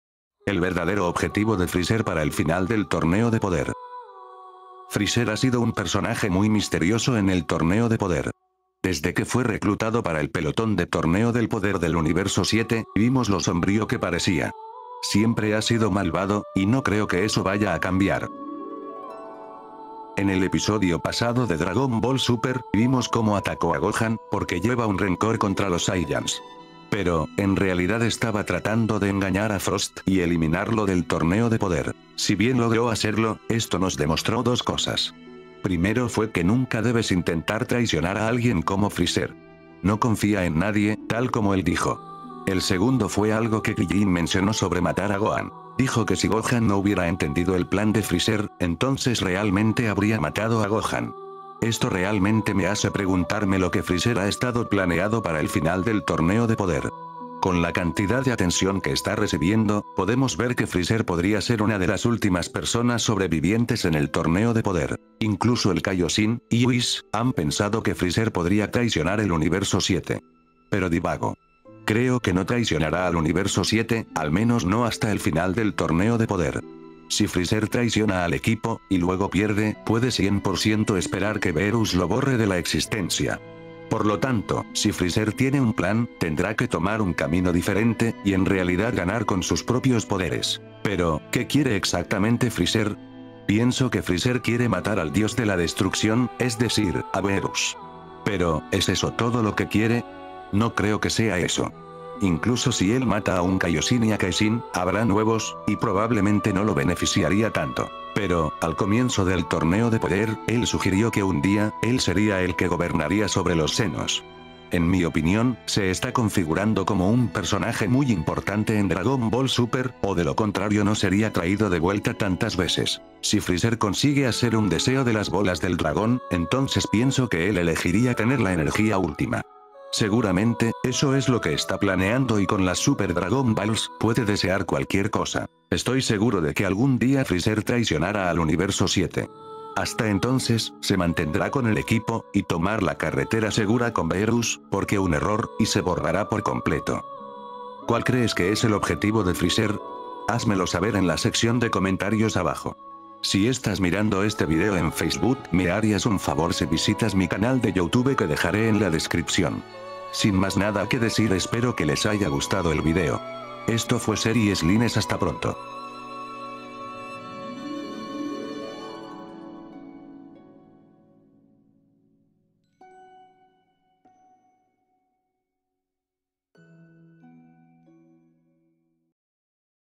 ¡El verdadero objetivo de freezer para el final del Torneo de Poder!
¡El verdadero objetivo de frizer para el final del Torneo de Poder!

frizer ha sido un personaje muy misterioso en el Torneo de Poder. Desde que fue reclutado para el pelotón de Torneo del Poder del Universo 7, vimos lo sombrío que parecía. Siempre ha sido malvado, y no creo que eso vaya a cambiar.

En el episodio pasado de Dragon Ball Super , vimos cómo atacó a Gojan, porque lleva un rencor contra los Saiyans. Pero, en realidad estaba tratando de engañar a Frost y eliminarlo del Torneo de Poder.

Si bien logró hacerlo, esto nos demostró dos cosas. Primero fue que nunca debes intentar traicionar a alguien como frizer. No confía en nadie, tal como el dijo. El segundo fue algo que Krillin mencionó sobre matar a Gohan.

Dijo que si Gojan no hubiera entendido el plan de frizer, entonces realmente habría matado a Gojan. Esto realmente me hace preguntarme lo que frizer ha estado planeado para el final del Torneo de Poder. Con la cantidad de atención que está recibiendo, podemos ver que frizer podría ser una de las últimas personas sobrevivientes en el Torneo de Poder.

Incluso el Kaioshin, y Whis, han pensado que frizer podría traicionar el Universo 7. Pero divago. Creo que  no traicionará al Universo 7, al menos no hasta el final del Torneo de Poder. Si frizer traiciona al equipo, y luego pierde, puede 100% esperar que Beerus lo borre de la existencia. Por lo tanto, si frizer tiene un plan, tendrá que tomar un camino diferente, y en realidad ganar con sus propios poderes.

Pero, ¿qué quiere exactamente frizer? Pienso que frizer quiere matar al Dios de la Destrucción, es decir, a Beerus. Pero, ¿es eso todo lo que quiere? No creo que sea eso. Incluso si él mata a un Kaioshin y Hakaishin, habrá nuevos, y probablemente no lo beneficiaría tanto.

Pero, al comienzo del Torneo de Poder, él sugirió que un día, él sería el que gobernaría sobre los Zenos. En mi opinión, se está configurando como un personaje muy importante en Dragon Ball Super, o de lo contrario no sería traído de vuelta tantas veces.

Si frizer consigue hacer un deseo de las bolas del dragón, entonces pienso que él elegiría tener la energía última. Seguramente, eso es lo que está planeando y con las Super Dragon Balls, puede desear  cualquier cosa.

Estoy seguro de que algun dia frizer traicionara al universo 7. Hasta entonces, se mantendrá con el equipo, y tomar la carretera segura con Beerus, porque un error, y se borrará por completo.

¿Cuál crees que es el objetivo de frizer? Házmelo saber en la sección de comentarios abajo!